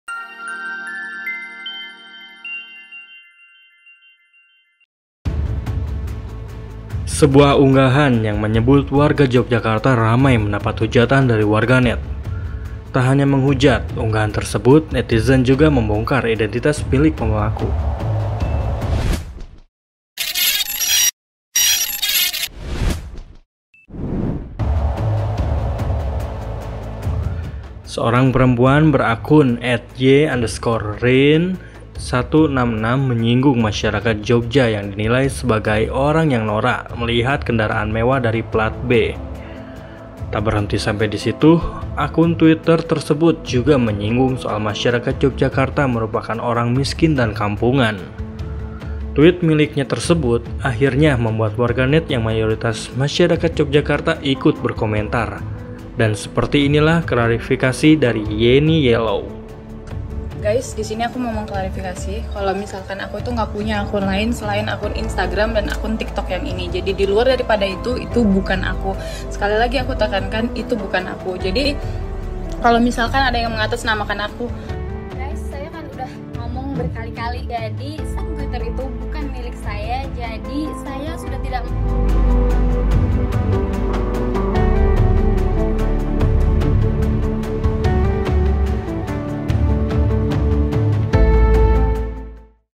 Klarifikasi Netizen Sebut Warga Jogja Miskin dan Kampungan
Sebuah unggahan yang menyebut warga Yogyakarta ramai mendapat kecaman dari warganet.

Tak hanya kecaman unggahan tersebut, netizen juga membongkar identitas pemilik akun.

seorang perempuan berakun @ye_riiin166, menyinggung masyarakat Jogja
yang dinilainya sebagai orang yang norak melihat kendaraan mewah dari plat B.